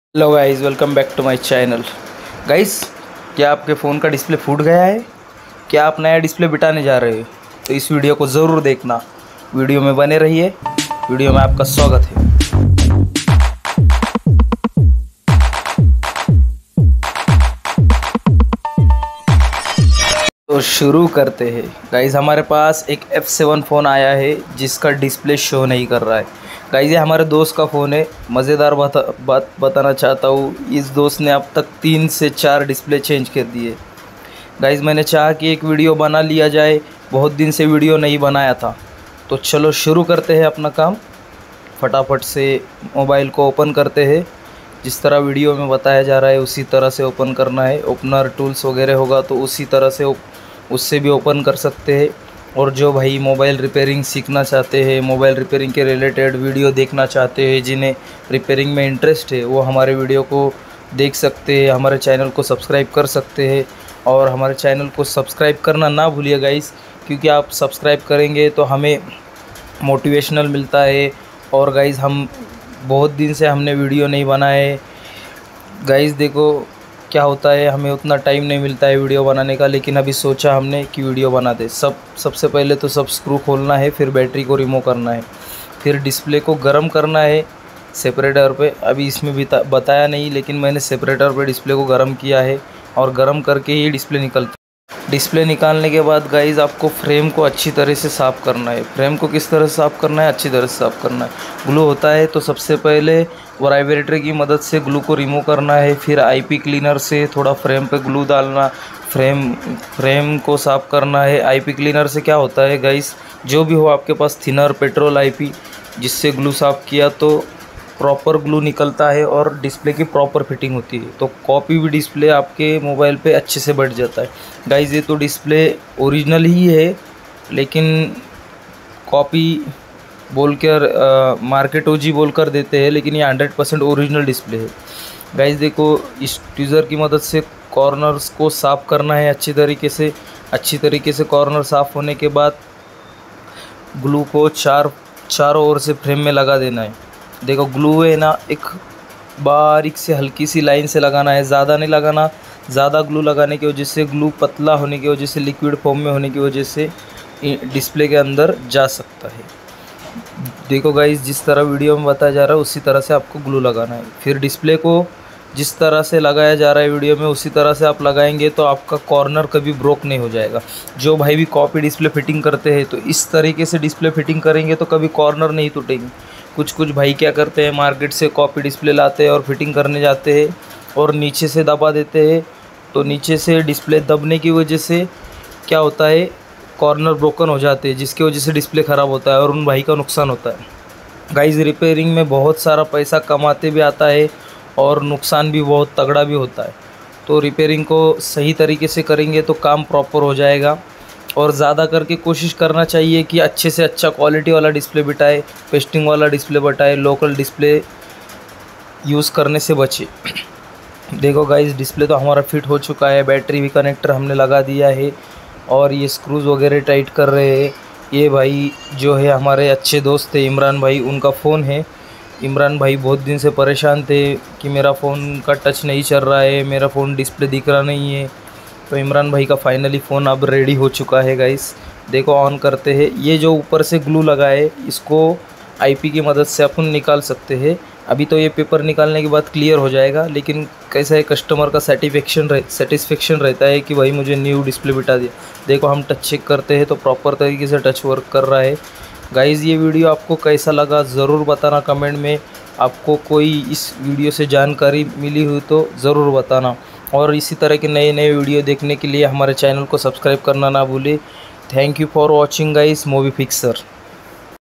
How To Fix Copy Display || क्या आप के फोन का डिस्प्ले खराब हुआ है तो इस वीडियो को पूरा जरूर देखें
How To Fix Copy Display ||
क्या आप के फोन का डिस्प्ले खराब हुआ है तो इस वीडियो को पूरा जरूर देखें ||
Easy to fix copy display
how to change display